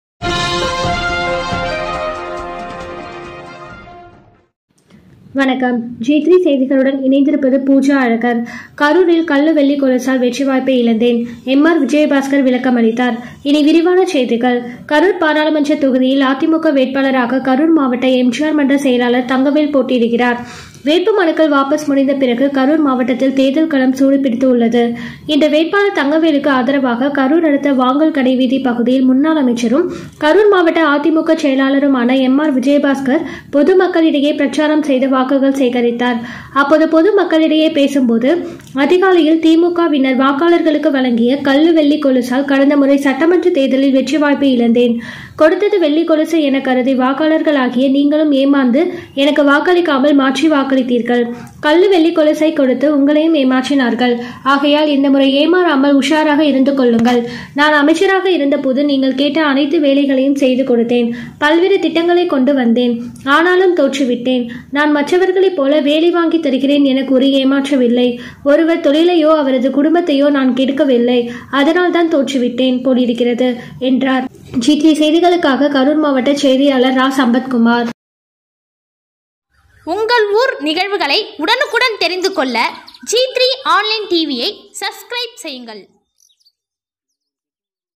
கல்லு வெள்ளி கொலுசால்..தோல்வியா...?
கரூர் நாடாளுமன்ற தொகுதிக்கு போட்டியிடும் தங்கவேலுவை ஆதரித்து எம் ஆர் விஜயபாஸ்கர் பொதுமக்கள் மத்தியில் வாக்குகள் சேகரித்தார்.